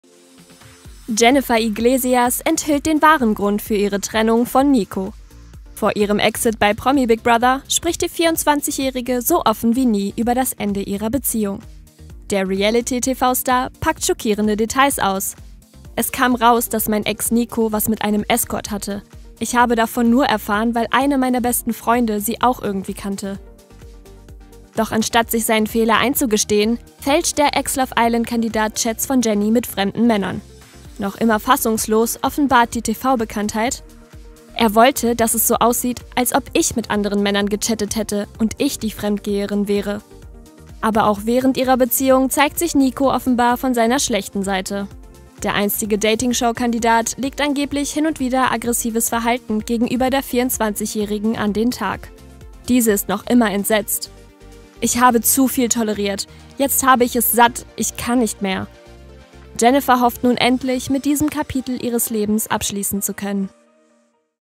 Jennifer Iglesias packt aus: Nico ging mit Escort-Dame fremd
Jennifer Iglesias (24) kann noch immer nicht glauben, was ihr widerfahren ist. Während ihrer Teilnahme an Promi Big Brother packte die einstige Love Island-Teilnehmerin nun über ihre Trennung von Nico aus. "Es kam raus, dass mein Ex Nico was mit einem Escort hatte. Ich habe davon nur erfahren, weil einer meiner besten Freunde sie auch irgendwie kannte”, offenbarte die Beauty gegenüber ihren Mitstreitern. Doch damit nicht genug – während ihrer Beziehung soll sich der ehemalige Datingshow-Kandidat ihr gegenüber aggressiv verhalten haben. "Ich habe zu viel toleriert", gab die TV-Bekanntheit zu. (swy/mji)